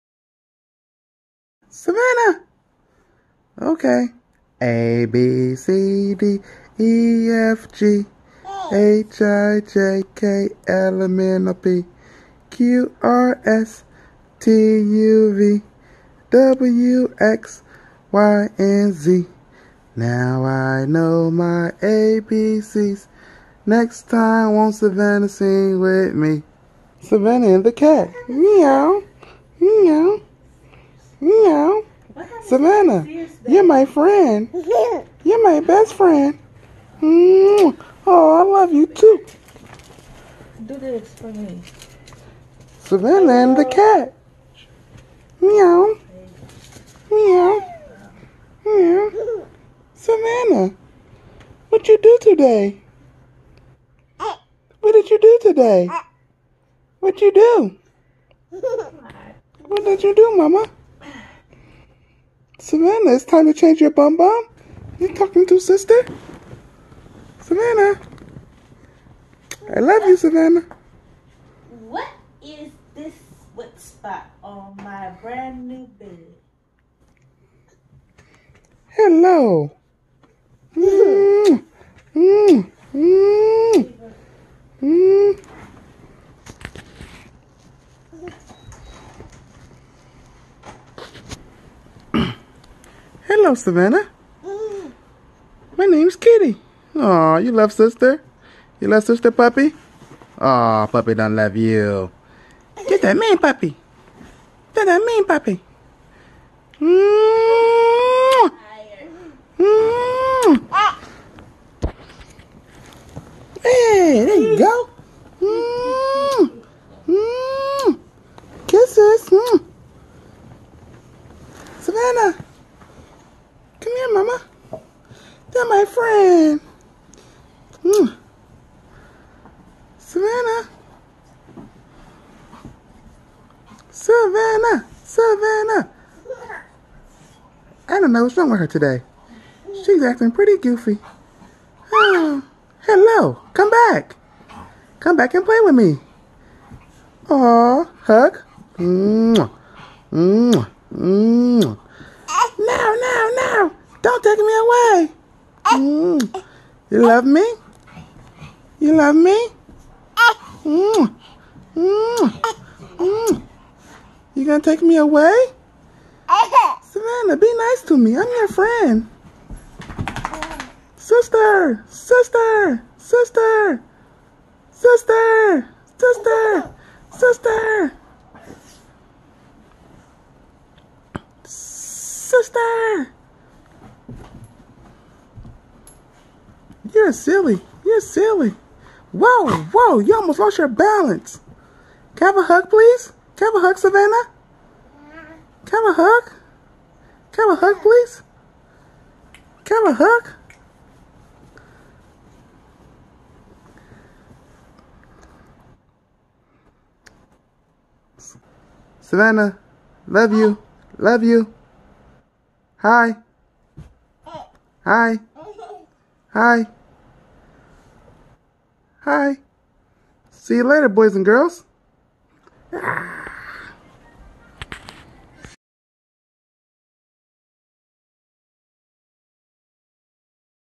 Savanna's Animals- Kitty Sister and Puppy Sister- ABC's
Savanna learns with all her puppet animal friends. Today features her kitty sister and puppy sister. ABC's